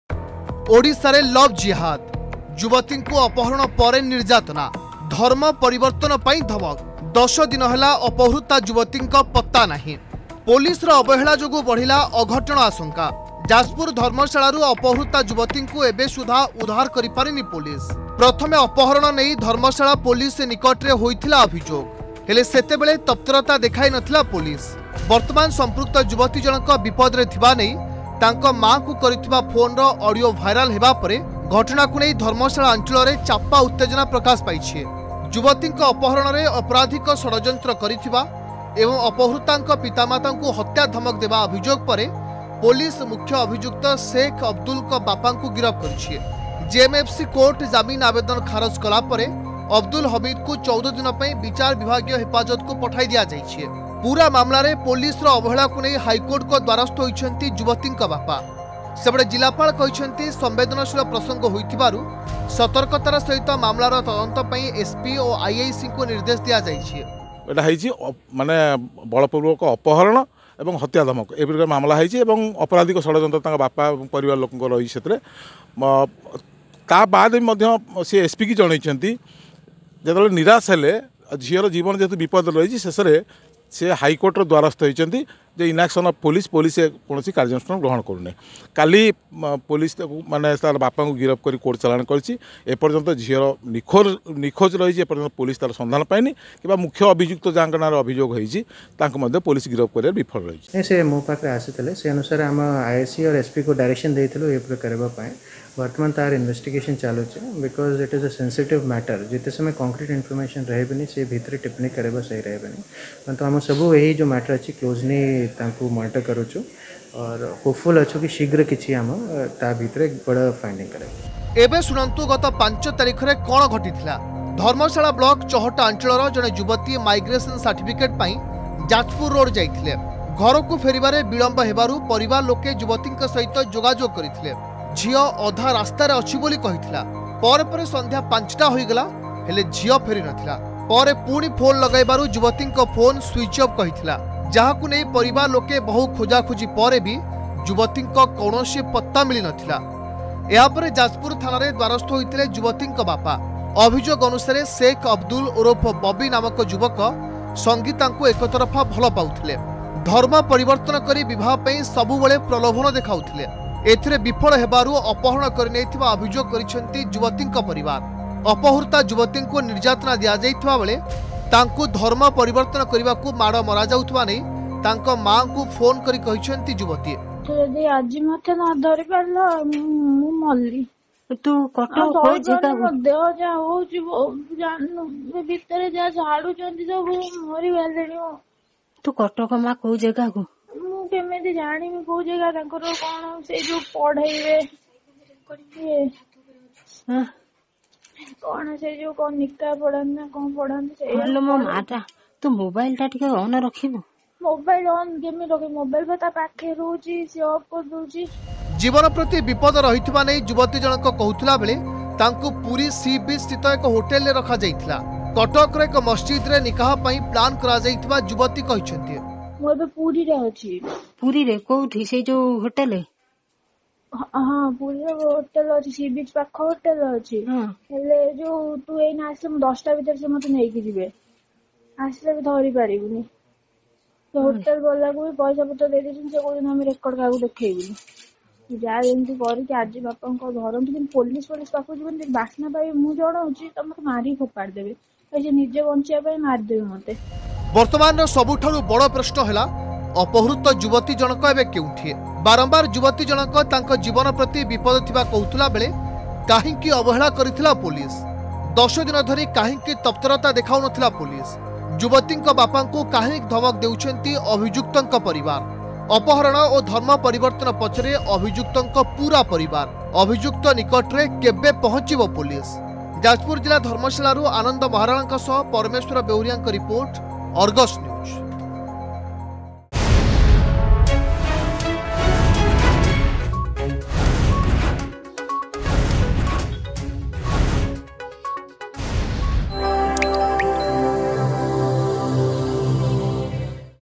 ଓଡ଼ିଶାରେ ଲଭ୍ ଜିହାଦ୍ ...
ଓଡ଼ିଶାରେ ଲଭ୍ ଜିହାଦ୍ I ଯୁବତୀଙ୍କୁ ଅପହରଣ ପରେ ନିର୍ଯାତନା  I I ଧର୍ମ ପରିବର୍ତ୍ତନ ପାଇଁ ଧମକ
10 ଦିନ ହେଲା ଅପହୃତ ଯୁବତୀଙ୍କ ପତ୍ତା ନାହିଁ...
ଅଧିକ ଖବର ପାଇଁ ଡ଼ାଉନଲୋଡ କରନ୍ତୁ “Argus News” ଆପ୍